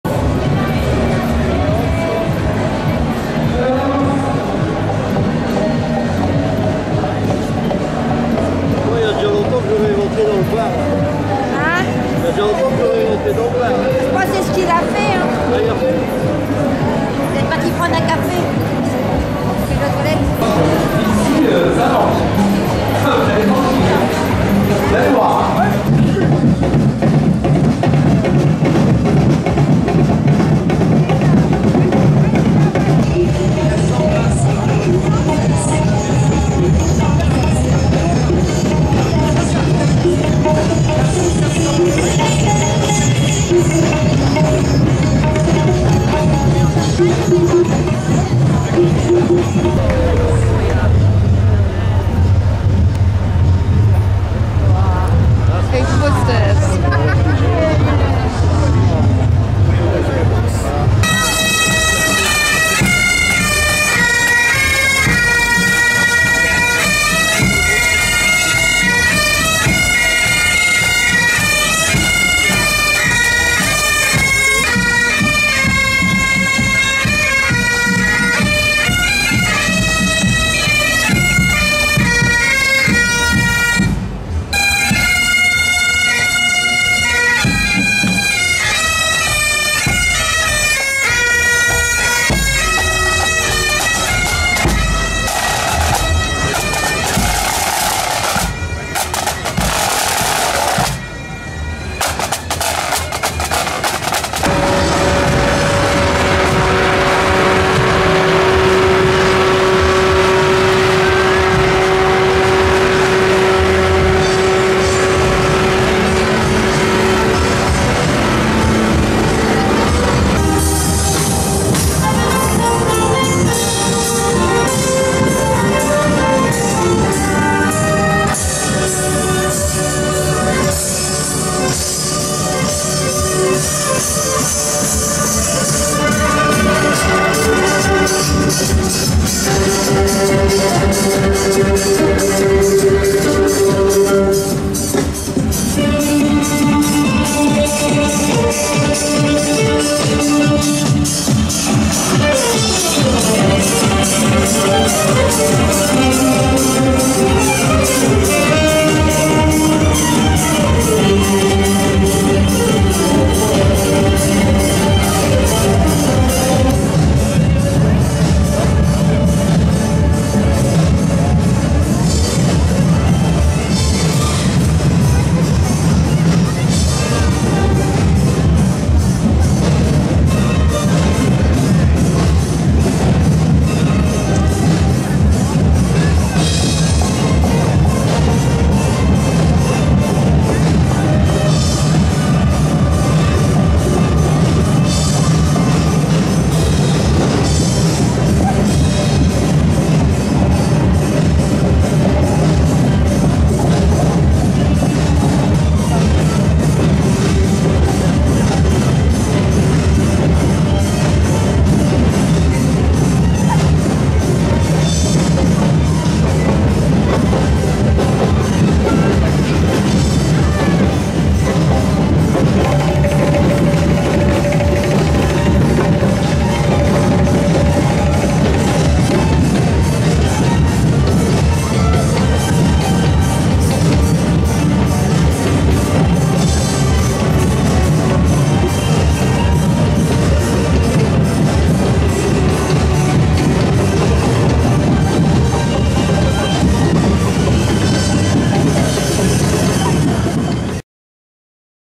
Lazurowe Wybrzeże, Menton, Święto Cytryny 02 2018
Święto cytryny w Menton na Lazurowym Wybrzeżu, luty 2018